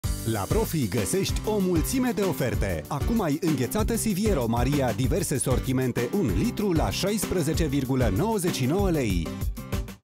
La Profi găsești o mulțime de oferte! Acum găsești Înghețată Siviero Maria 1L la un pret special.
La Profi găsești o mulțime de oferte! Acum găsești Înghețată Siviero Maria fructe de pădure, vanilie, fistic sau 3 feluri de ciocolata 1L la un pret special. Oferta supusa unor termene si conditii. Profi. Zilnic preturi mici.